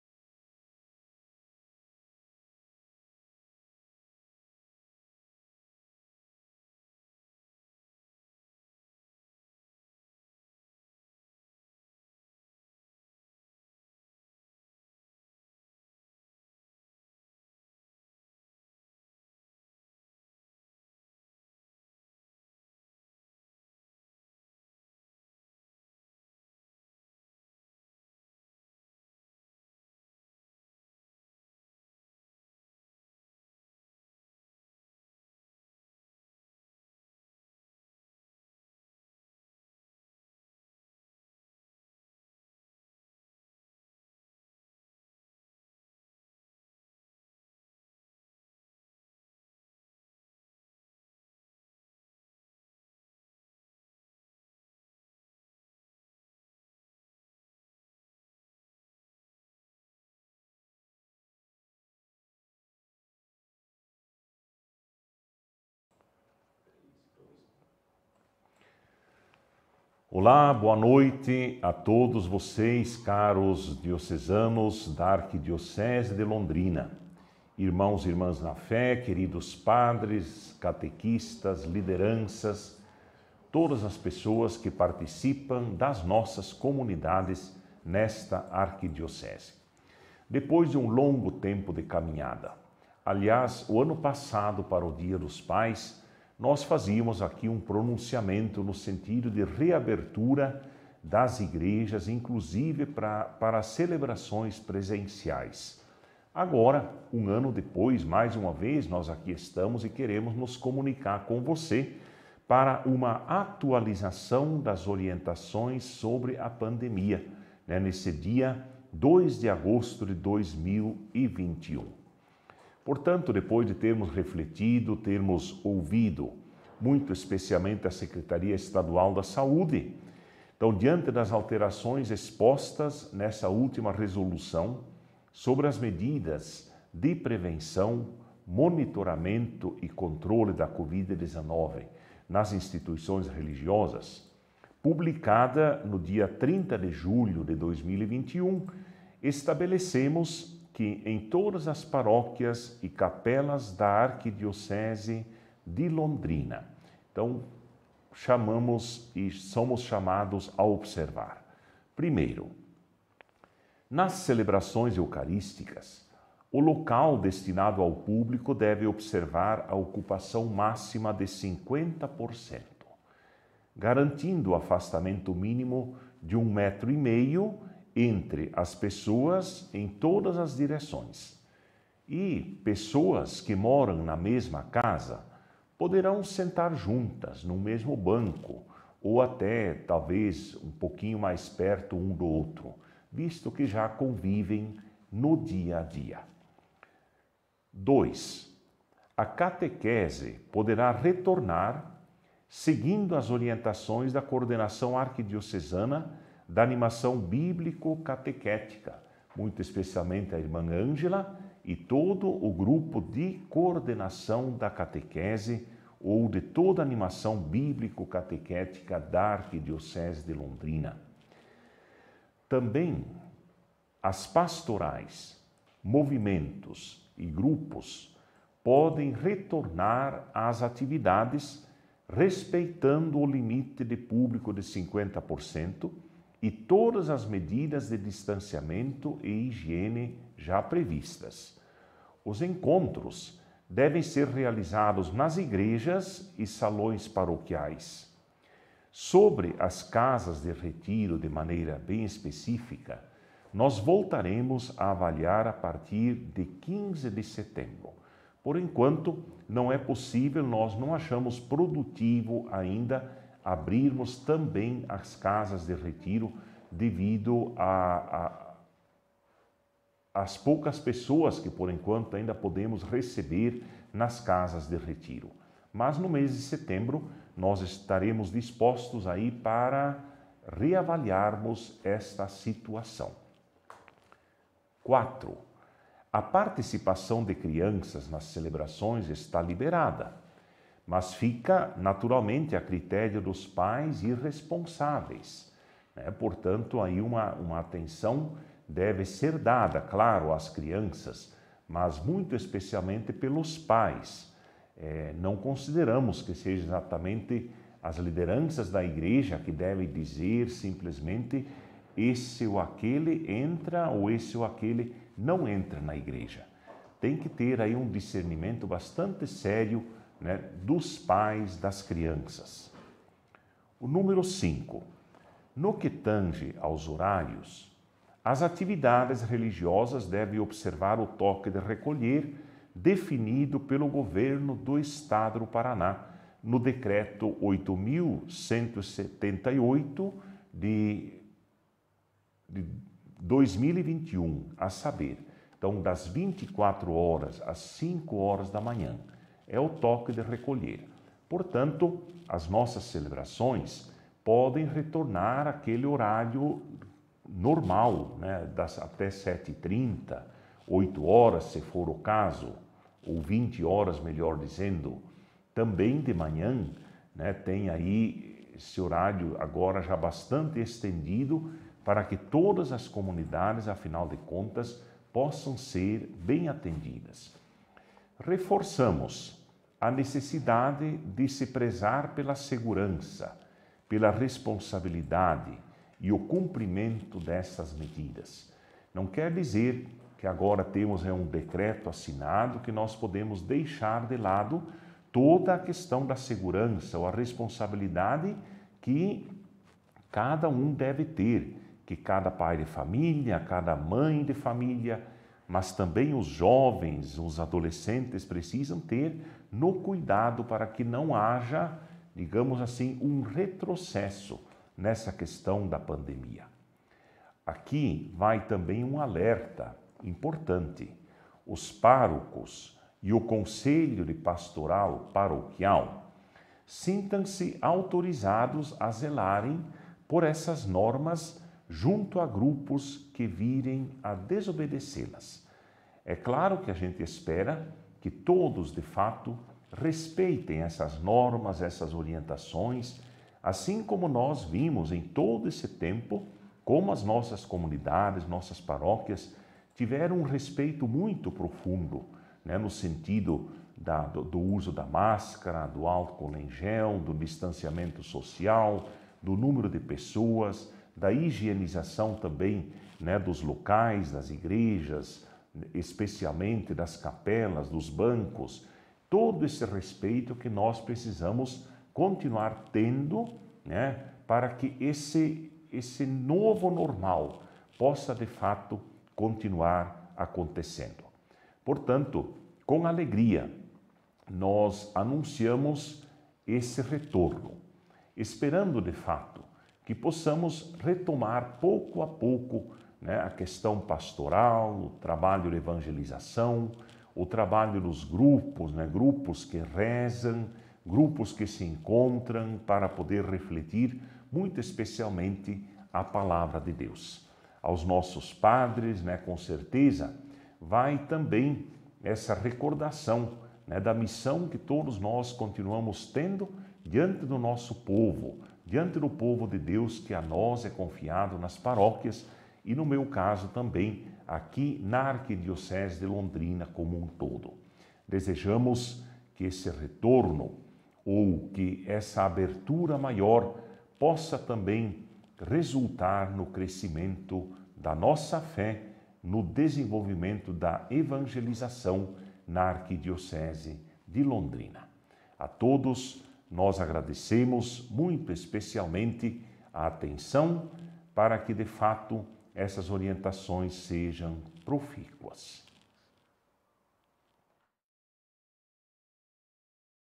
Atualização das orientações sobre a pandemia - 2/8/2021
O arcebispo dom Geremias Steinmetz divulga novas orientações para as atividades nas paróquias e capelas da Arquidiocese de Londrina.